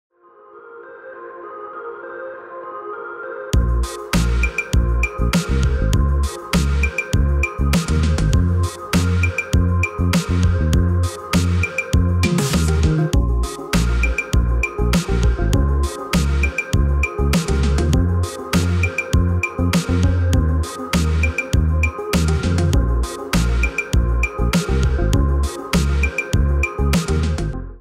Распаковка сборной модели IA 58 Pucara от производителя KINETIC.
Масштаб - 1:48